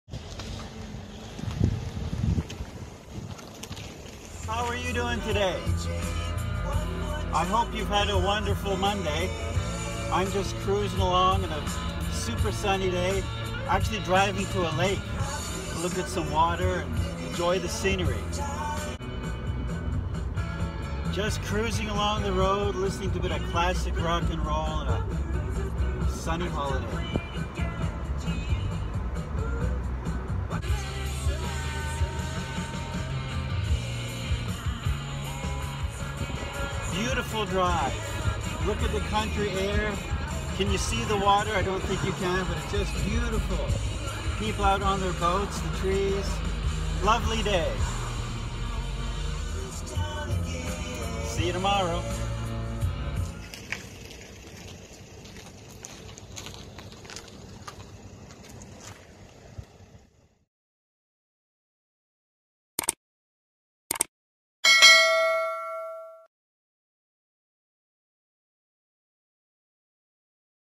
Monday Drive With The Top Down
Purposeless drive on a sunny Monday in a Mercedes-Benz SL